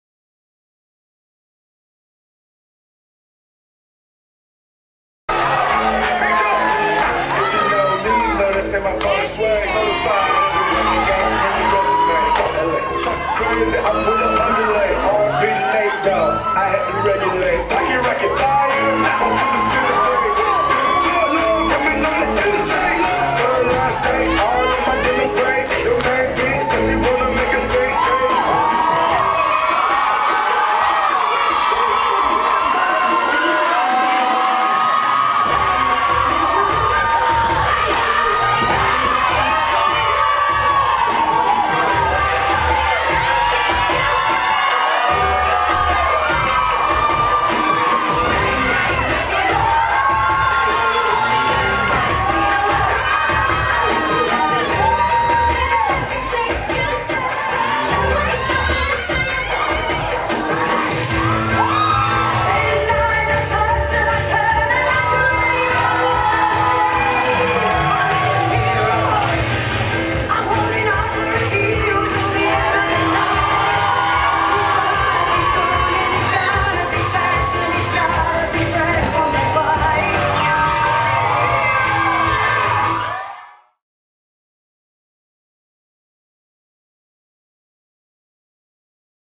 Homecoming King and Queen Opening Performance
Meet the finalists for the 2014 Homecoming King and Queen Superhero Showdown